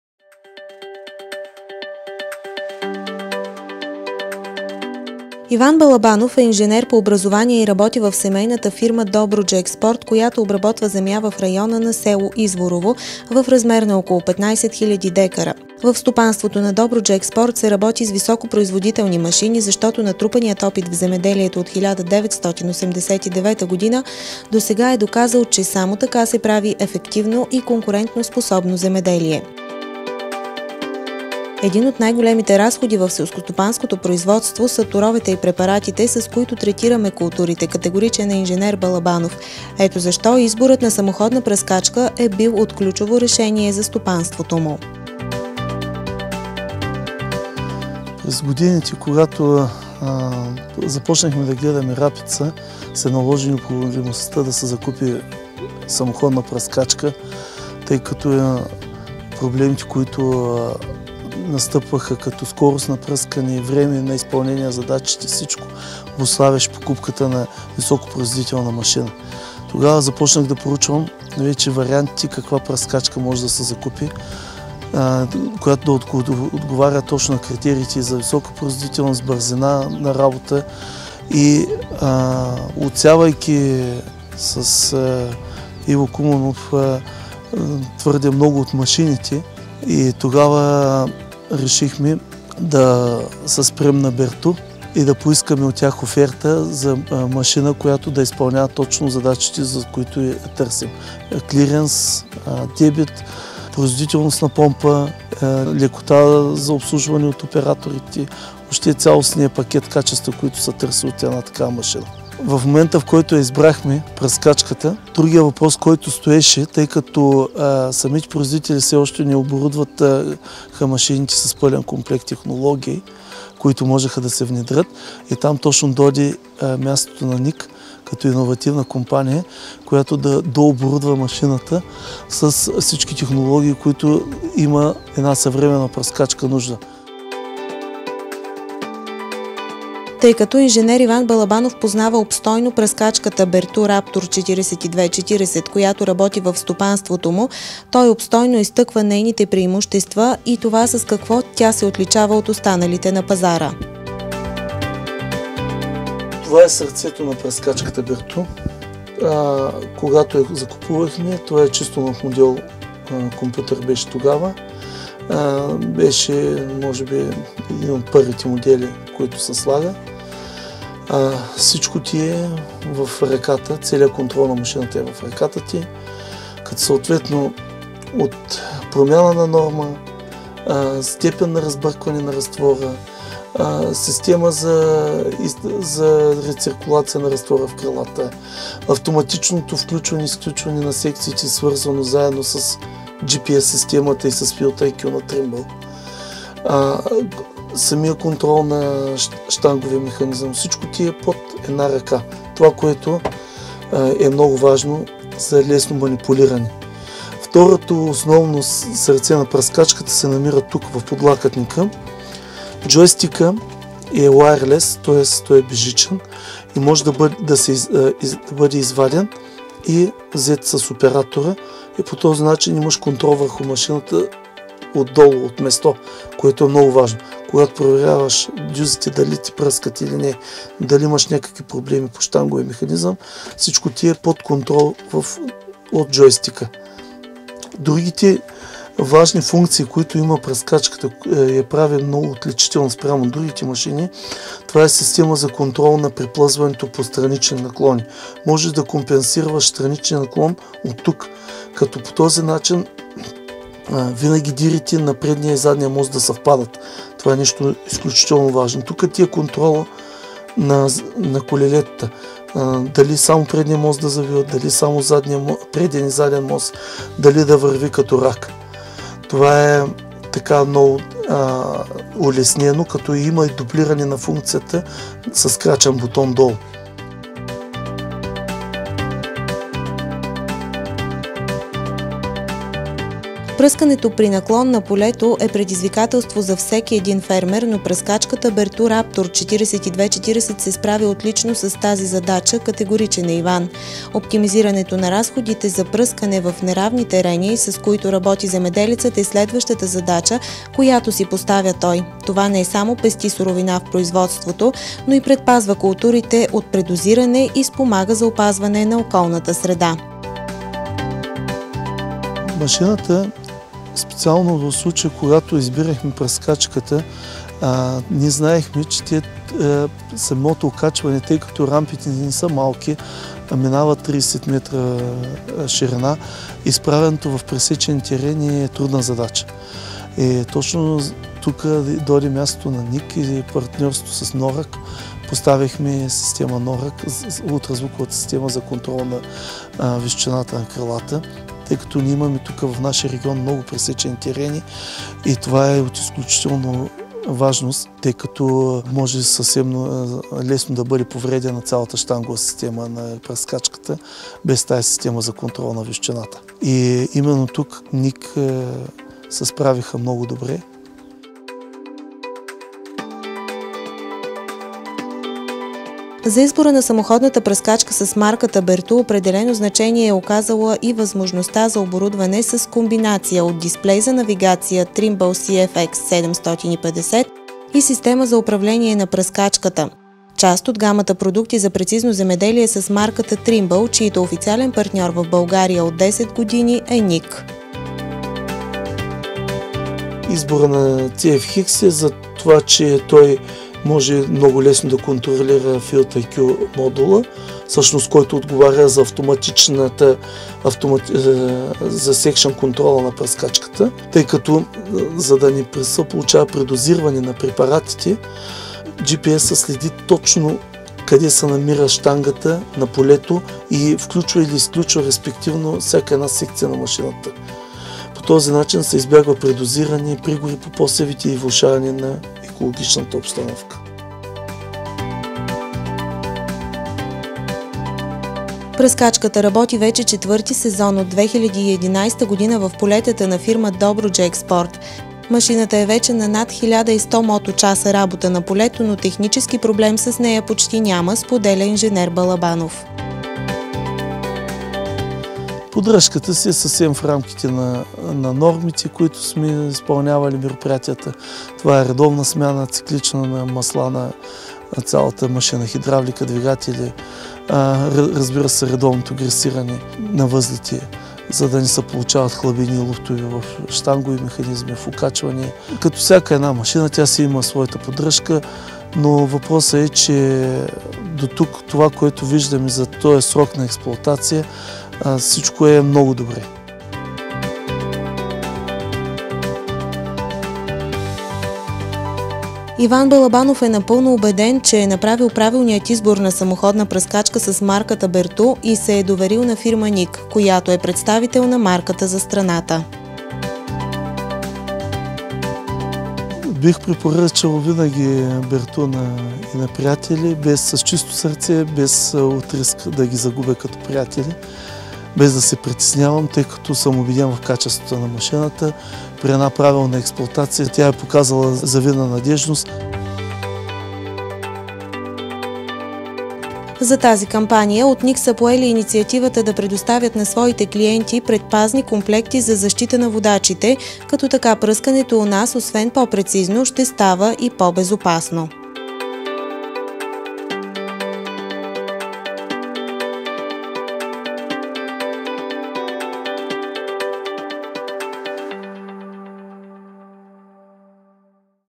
Berthoud Raptor в действие!
Фермер от Добричко споделя за дългогодишния си опит със самоходната пръскачка Berthoud Raptor.